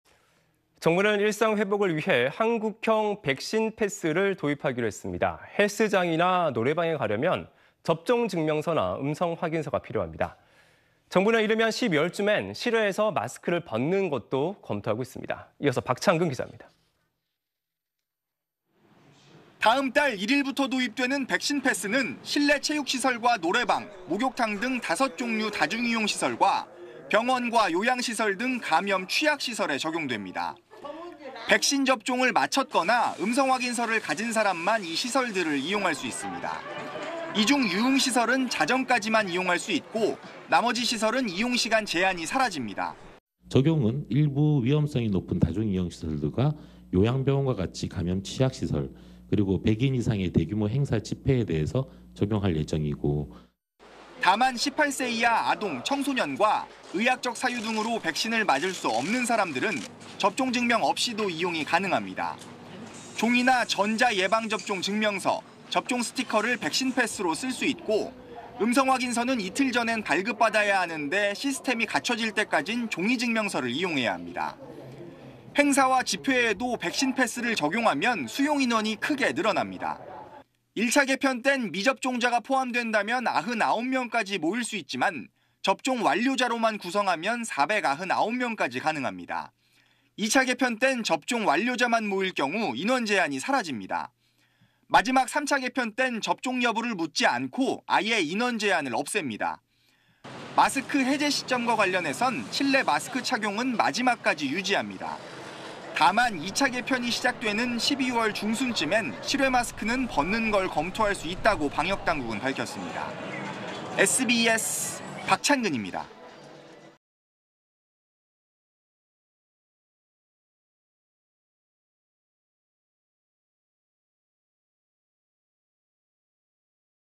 헬스장 · 노래방선 '백신 패스'…12월 실외 마스크 벗나 / SBS
정부는 일상 회복을 위해 한국형 백신 패스를 도입하기로 했습니다. 헬스장이나 노래방에 가려면 접종증명서나 음성확인서가 필요합니다. 정부는 이르면 12월쯤에는 실외에서 마스크를 벗는 것도 검토하고 있습니다.
이어서, 박찬근 기자입니다.